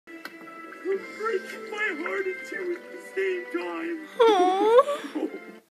💔💔💔💔💔💔💔💔💔💔💔 my reaction to YouTube video editor removed 💔💔💔💔💔💔💔💔💔💔💔💔💔💔💔💔
Now I'll never make anymore crossover videos and more random comic madness episodes! 😭😭😭😭😭😭